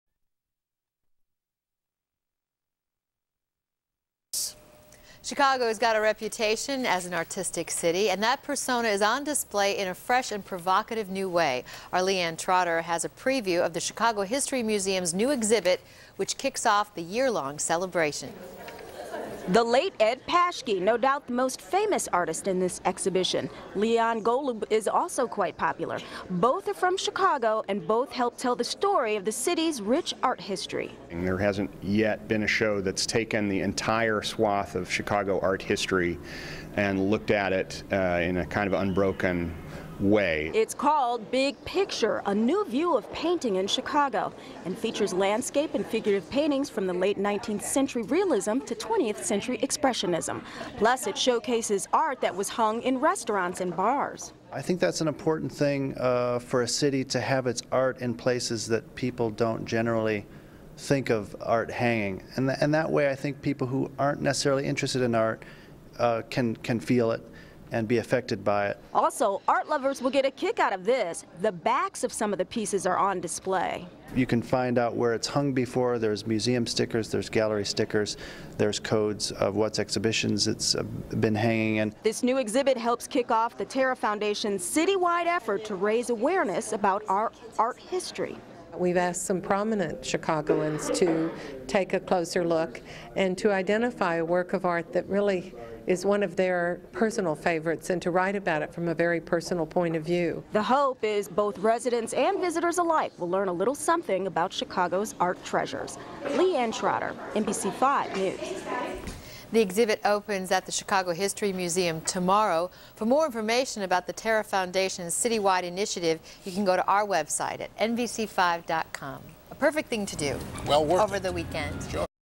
Big Picture on NBC5
NBC 5 story on BIG PICTURE: A NEW VIEW OF PAINTING IN CHICAGO at the Chicago History Museum, October 2007. Curated by John Corbett and Jim Dempsey